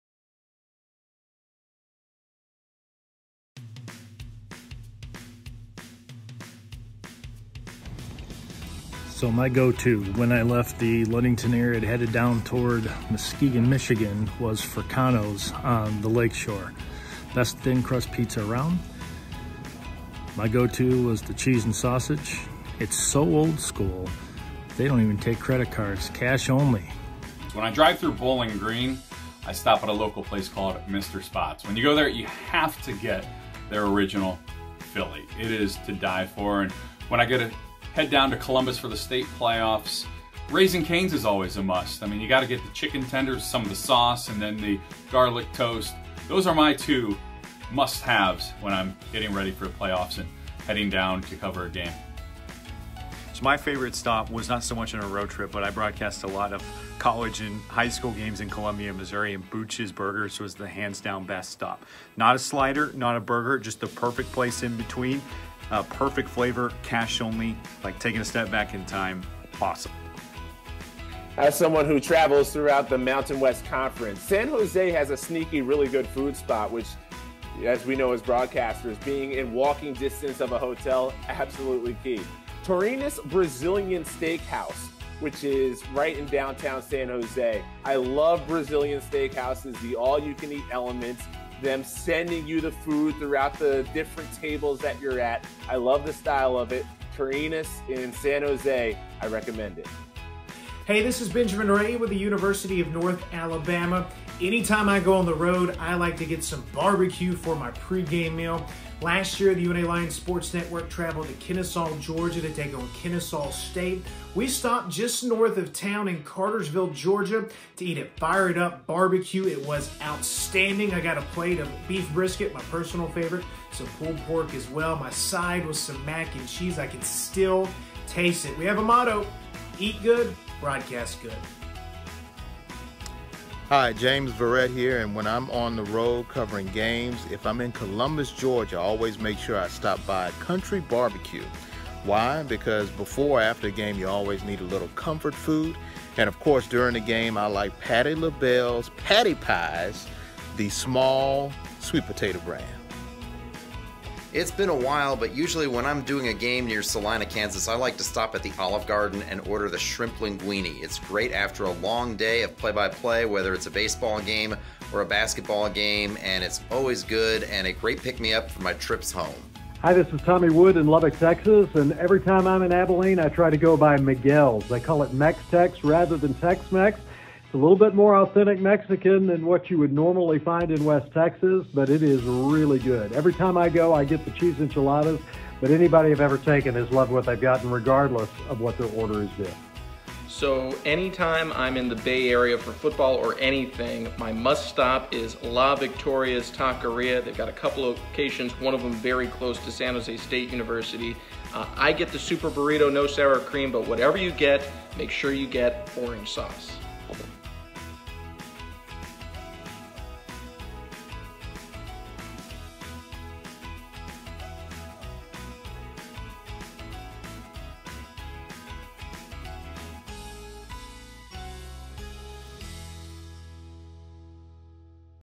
Sportscasters' Favorite Restaurants on the Road
What are your favorite food spots when you're broadcasting on the road?